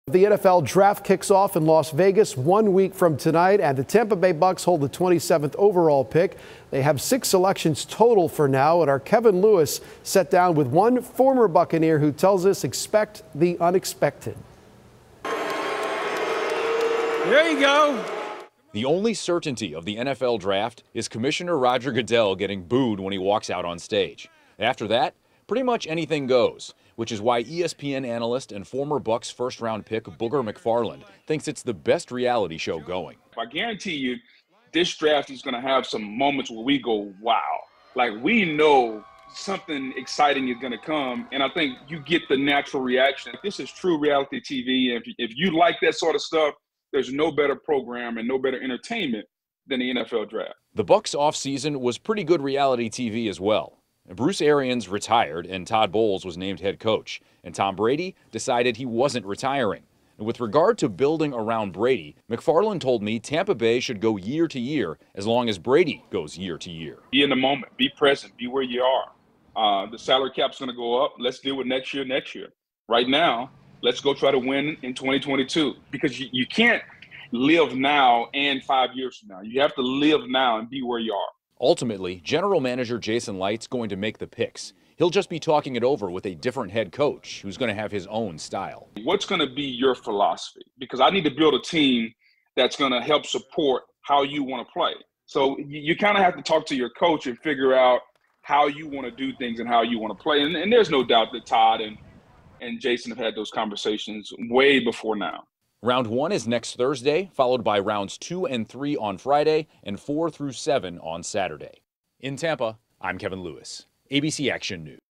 Former Buccaneer, ESPN analyst Booger McFarland says expects the unexpected in the 2022 NFL Draft
Day one of the NFL Draft kicks off Thursday, April 28 in Las Vegas. Former Bucs first round pick and current ESPN analyst Booger McFarland says it's the best reality show on TV.